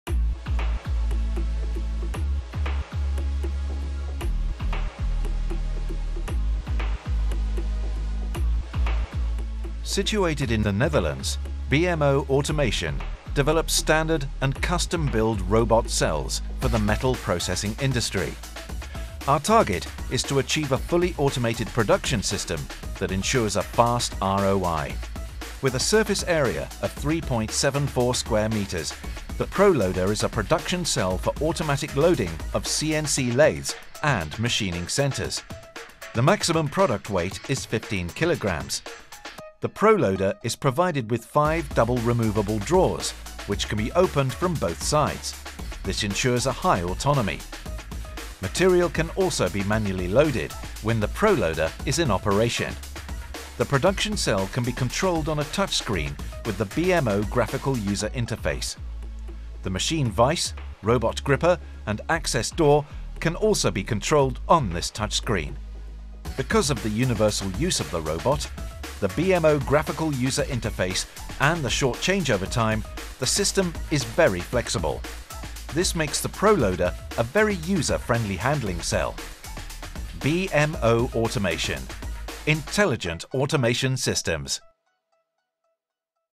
BMO Pro-loader robotsystem
The Pro-loader robotsystem is a user-friendly handling system for CNC machines in the metal processing industry.